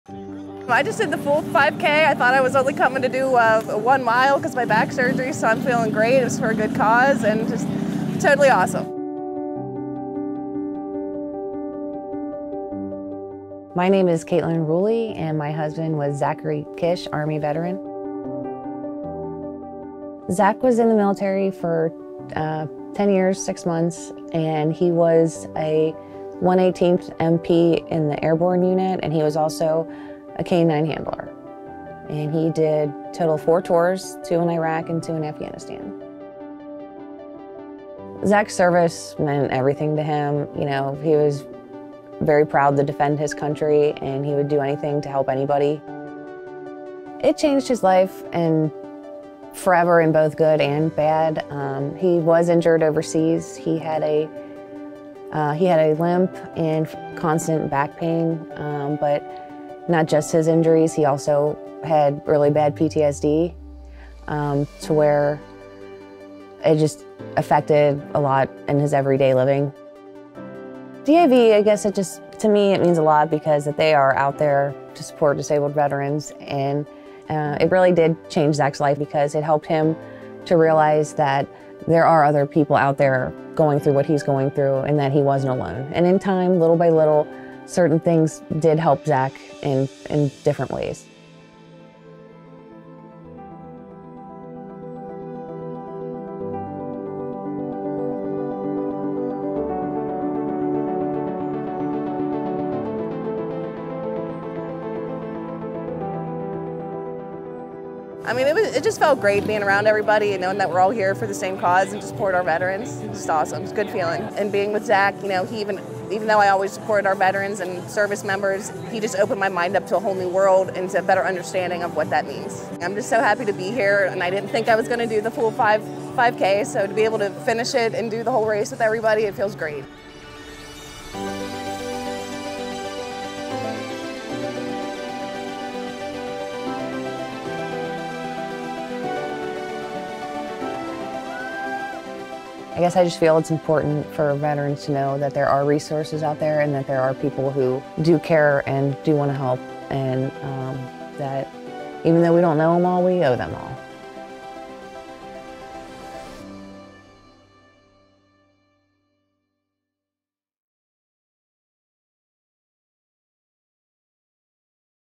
DAV5K - Caitlin Rulli
Caitlin Rulli participated in the 2022 DAV 5K in honor of her husband Zach, who passed away in 2020. Zach was an Army veteran who served in Iraq and Afghanistan. After having back surgery, Caitlin was unsure whether she would be able to complete the full 5K, but she persevered.
Thanks to everyone who took part in the DAV 5K this year for the 10th anniversary! Learn more and check back for updates about the 2023 5K